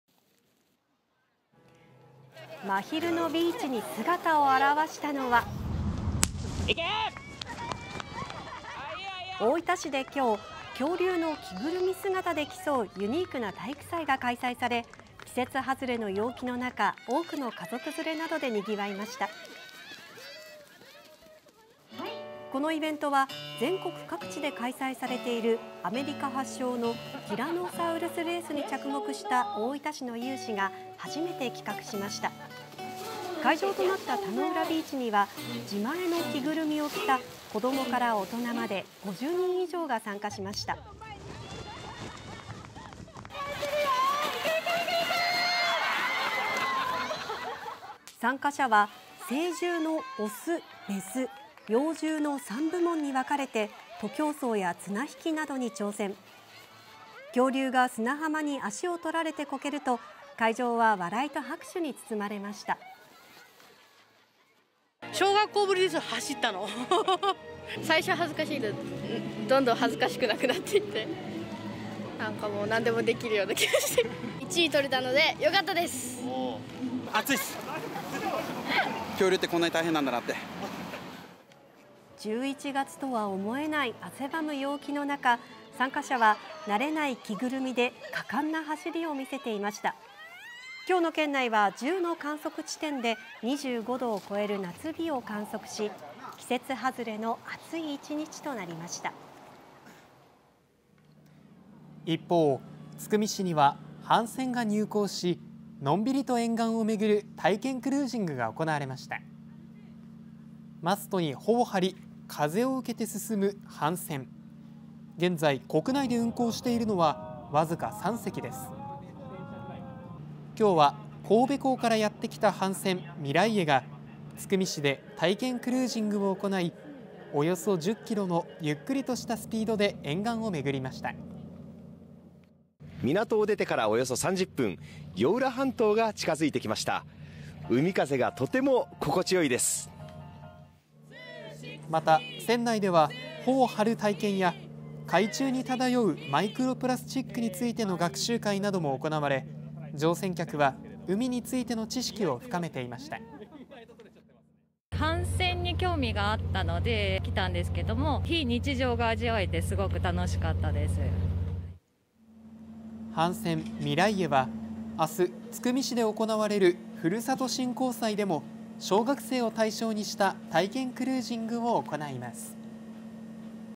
「恐竜の体育祭」「帆船クルージング」 季節外れの陽気…連休初日にぎわう 大分
大分市で恐竜の着ぐるみ姿で競うユニークな体育祭が開催され、季節外れの陽気のなか、多くの家族連れでにぎわいました。